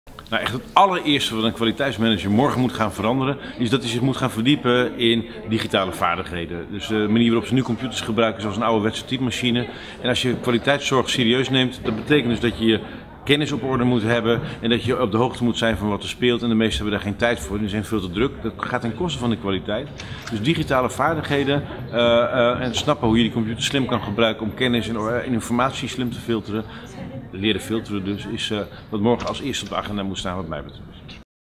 19e Nationaal Kwaliteitscongres 2015 - Martijn Aslander
Hoor wat Martijn Aslander vindt wat u morgen moet veranderen als kwaliteitsmanager!